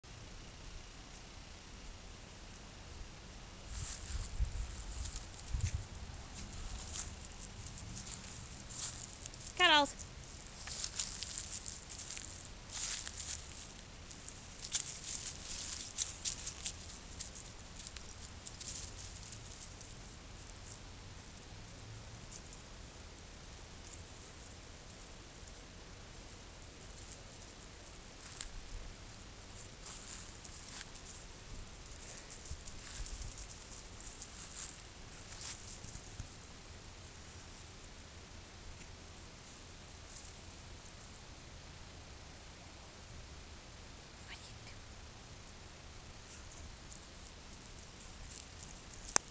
Cuddles
Cuz of her iris atrophy she has trouble seeing in the sunlight.